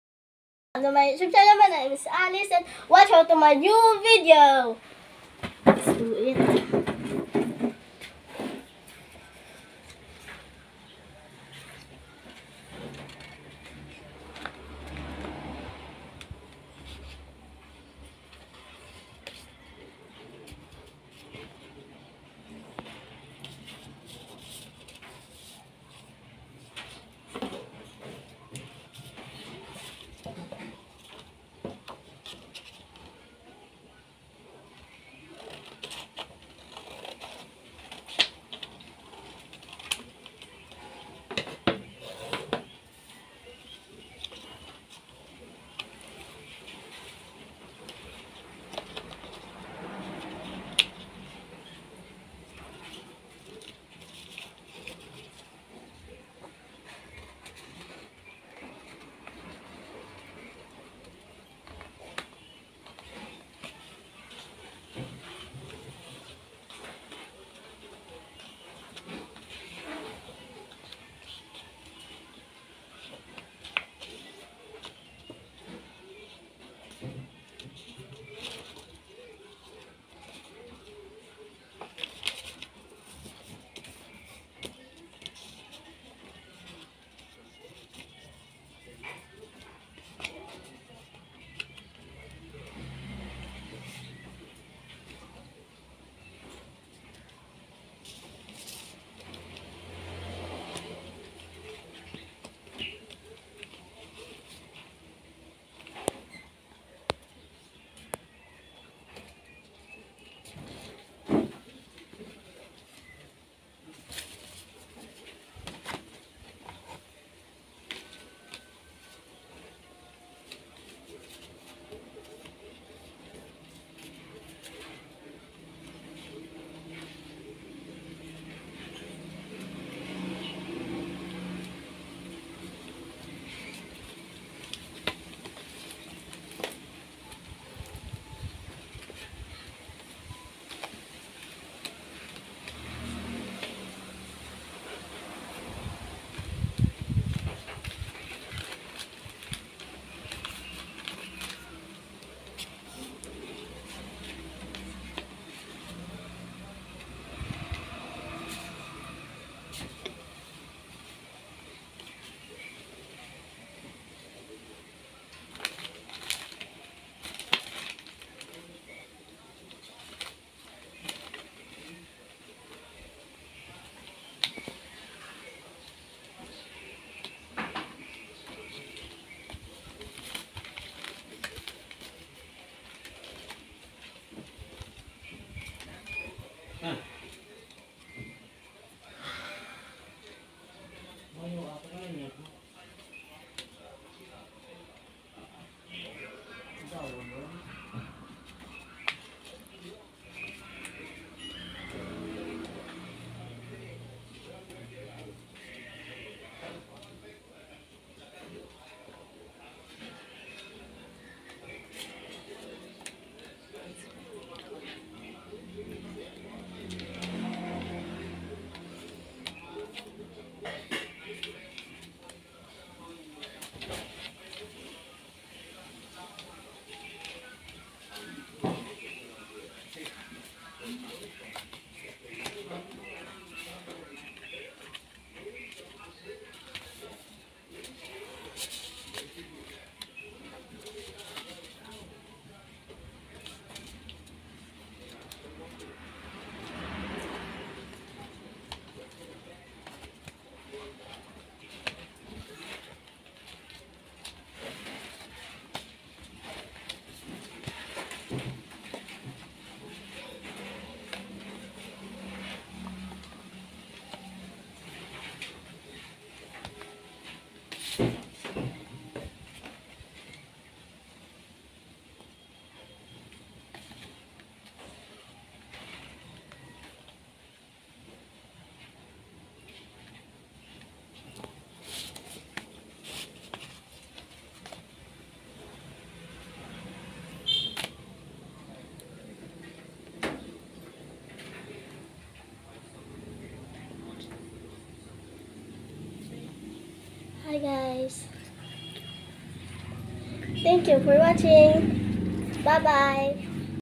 Paper envelope making by Twin Alice the little vlogger
How to make a paper  envelope by Twin channel buzz @linnschannel7024